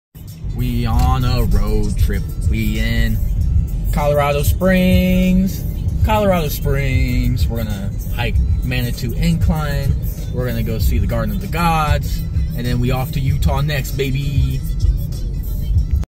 Headed to Colorado Springs for an adventure! 😁
The wife and I head to Colorado Springs, CO to see some sites and go hiking!